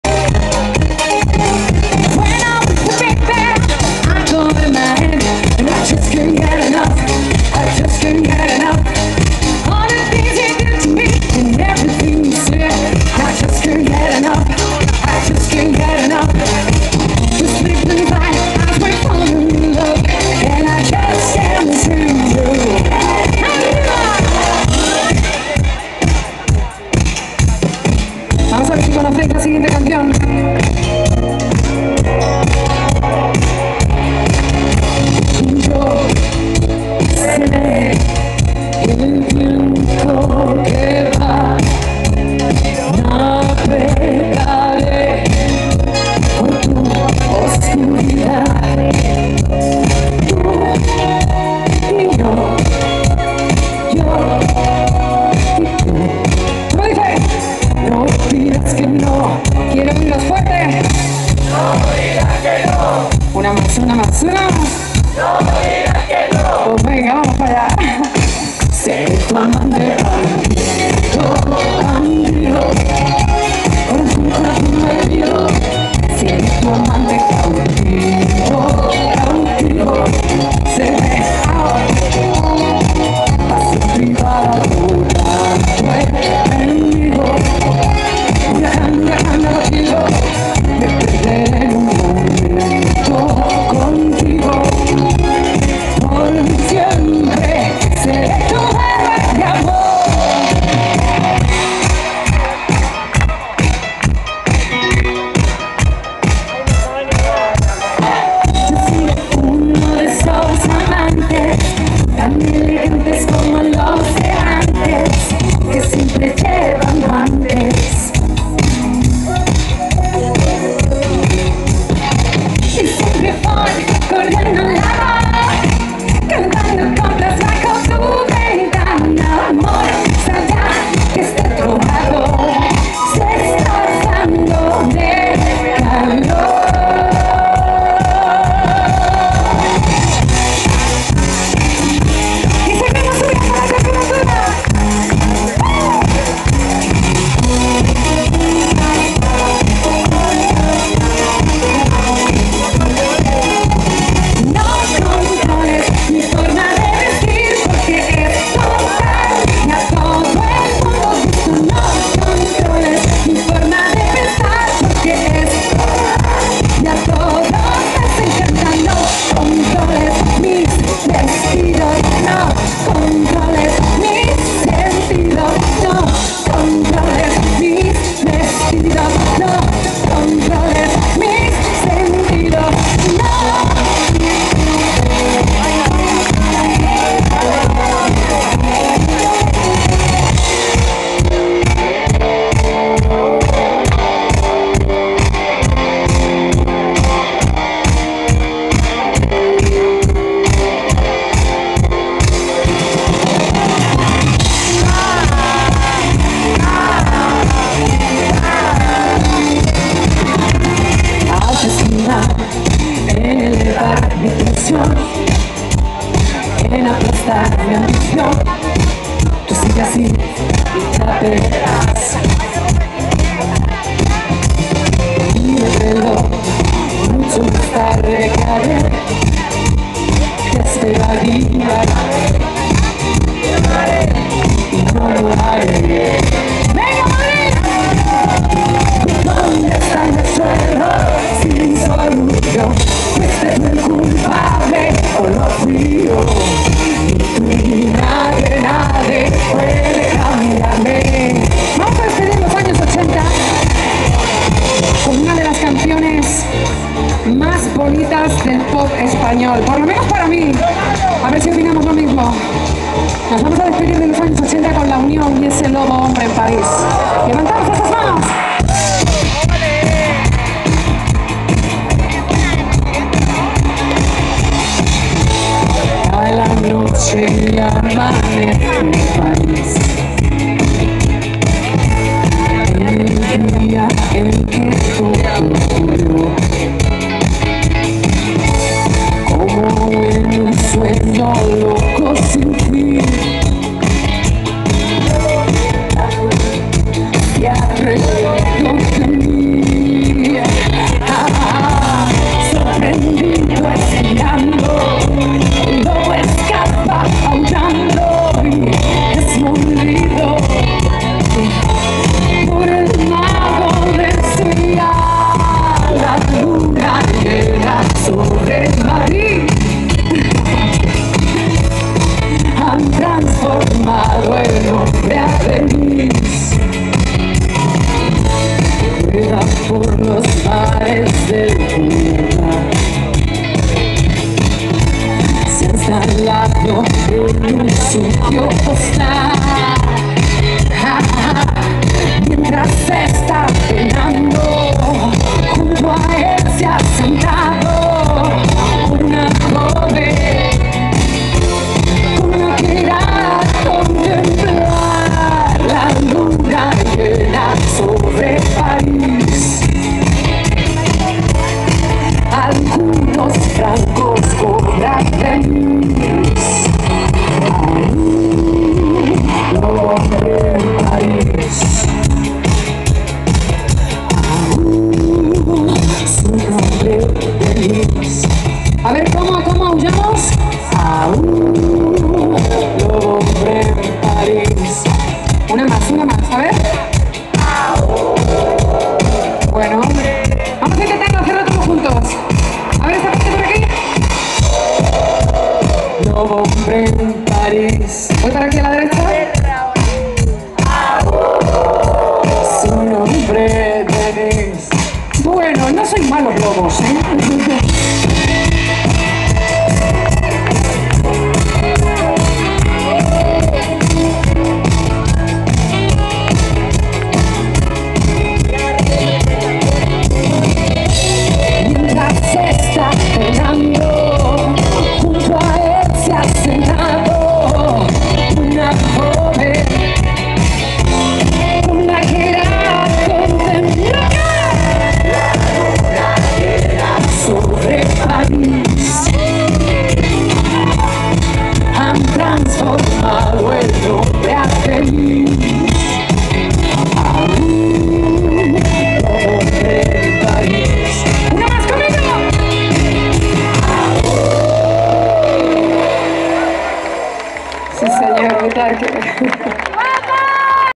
Soraya (Vídeo 5)
Concierto en Butarque (Villaverde) el día 23/06/2018